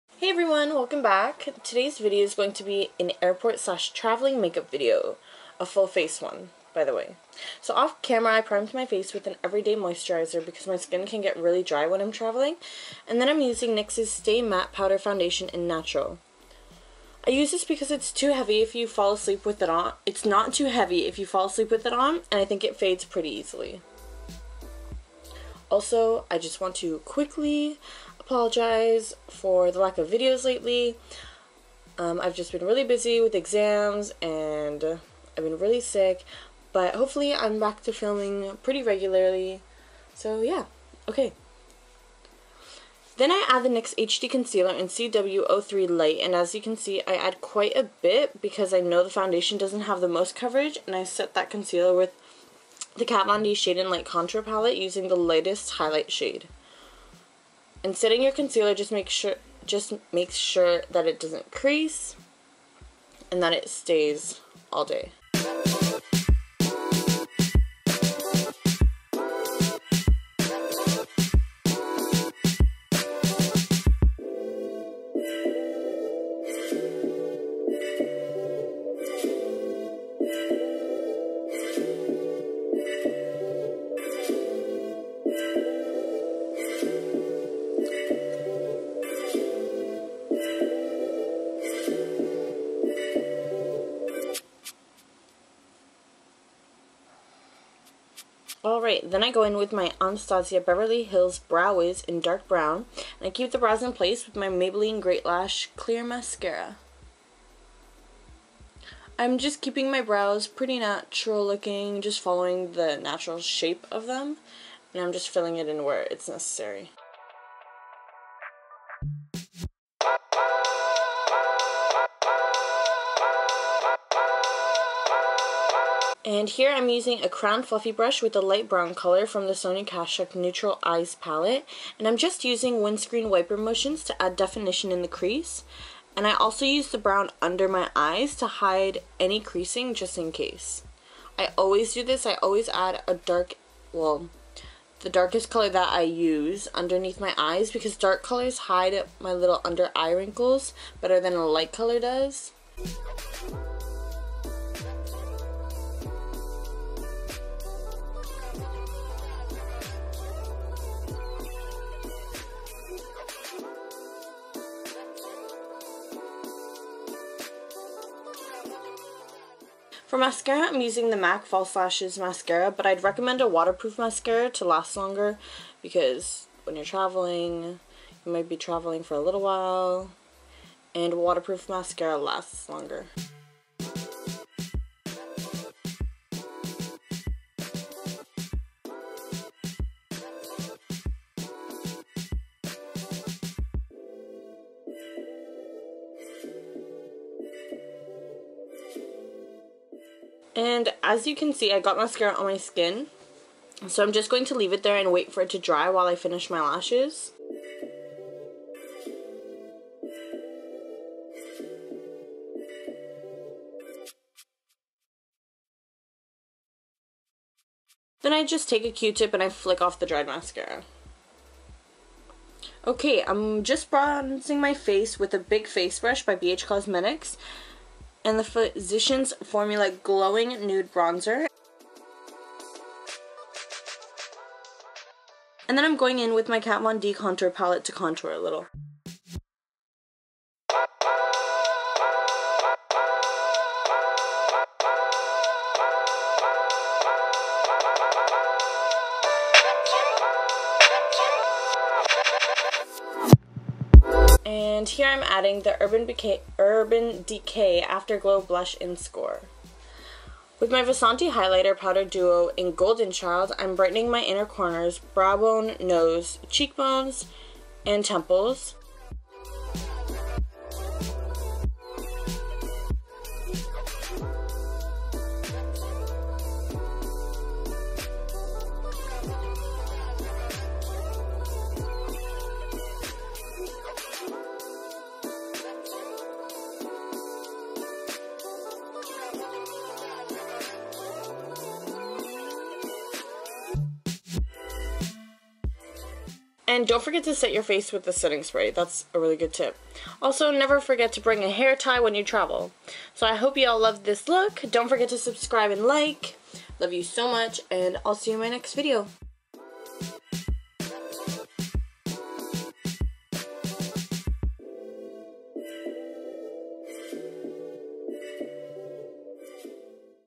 Full Face Airport & Travelling Makeup | SAMIAMBEAUTY
Hey everyone!! Thank you so much for stopping by my channel! Sorry again for the delay in videos but I just recently got better from being sick for so long and I also just finished exams, so now my time is more freed up :) I hope you all enjoy this full face traveling/airport makeup look! This is a look I have done recently, when I wanted to put a little bit of effort and not appear so tired.

Music is a J. Cole remix by a friend of mine- Check out his channel here on YouTube: TROOLY

Makeup Details:
Foundation: Nyx Stay Matte but not Flat Powder Foundation in Natural
Concealer: Nyx HD Concealer in CW03 Light
Highlight/Contour: Kat Von D Shade & Light Contour Palette
Brows: Anastasia Beverley Hills Brow Wiz in Dark Brown & Maybelline Great Lash Clear Mascara
Eyes: Sonia Kashuk Eyes on Neutral Palette
Lashes: Mac False Lashes Mascara
Bronzer: Physicians Formula Glowing Nude Bronzer
Illuminator: Vasanti Cosmetics Highlighter Duo in Golden Child
Brushes: BH Cosmetics, Crown Brushes